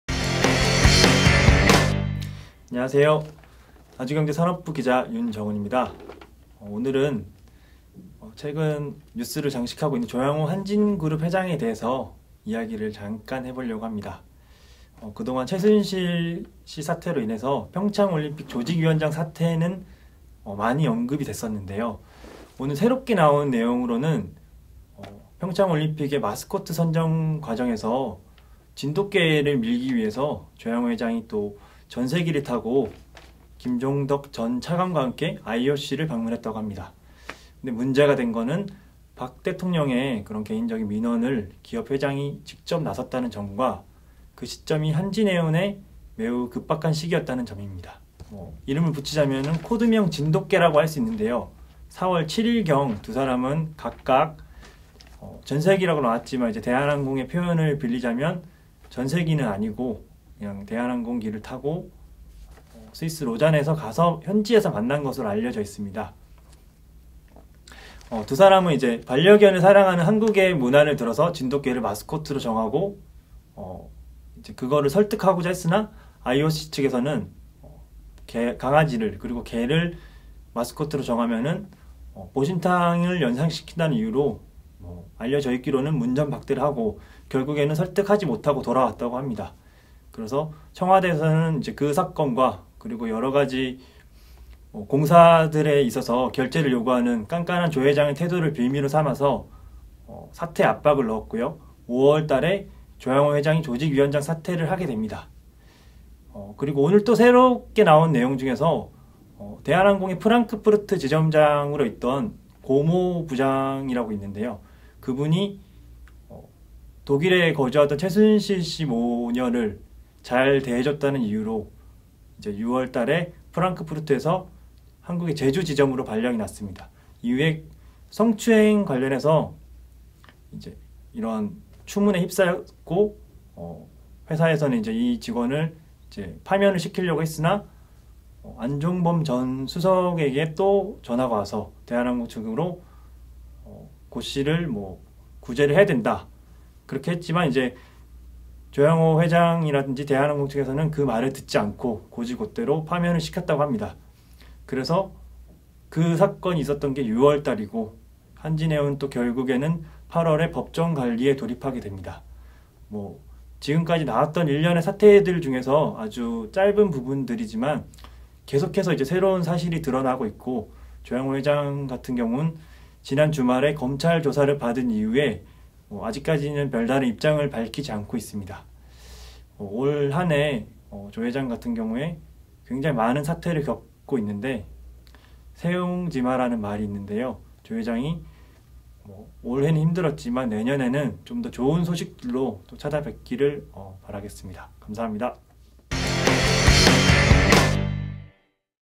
한진해운 법정관리 이모저모 ‘조양호 회장의 새옹지마’
한진해운 법정관리 이모저모

안녕하십니까 아주경제 산업부 기자 윤정훈입니다.

오늘도 조양호 한진그룹 회장에 관한 뉴스로 떠들썩했는데 잠깐 살펴보려고 합니다. 그동안 최순실 사태로 인해서 평창올림픽 조직위원회 사퇴까지는 나왔던 이야기인데, 새롭게 나온 내용은 마스코트 선정 건으로 조 회장이 IOC를 방문했다는데 있습니다.

문제가 된 건 박 대통령의 민원 해결을 위해 기업 회장이 나섰다는 점과 그리고 시점이 한진해운으로 인해서 매우 급박한 시기였다는 것입니다.

이름을 붙이자면 '코드명 진돗개'라고 할 수 있습니다. 4월7일 김종덕 전 장관과 조양호 회장은 스위스 로잔으로 떠난 것으로 알려져 있습니다. 두 사람은 반려견을 사랑하는 한국의 문화를 들어 진돗개를 마스코트로 하려고 했으나 IOC에서는 보신탕을 연상시킨다는 이유로 거절했다고 합니다.

그리고 조양호 회장은 5월 조직위원장을 사퇴하고, 한진해운은 조건부 자율협약에 들어갑니다.

오늘 또 새롭게 보도한 내용은 대한항공의 프랑크푸르트 지점장 고 씨입니다. 고 씨는 최순실의 편의를 잘 봐준 것으로 알려졌습니다. 그리고 이 고 씨는 이후 사내 성추행에 휘말렸고, 안종범 전 수석의 두 번째 전화가 갑니다. 하지만 대한항공은 고 씨를 원칙대로 처리합니다. 그리고 8월 한진해운의 법정관리에 들어갑니다.

조양호 회장은 지난 주말 검찰 조사 이후 입장을 밝히지 않고 있습니다. 새옹지마라고 합니다. 올해 유난히 힘든 일이 많은 조양호 회장, 내년에는 좋은 소식을 많이 들려주셨으면 합니다.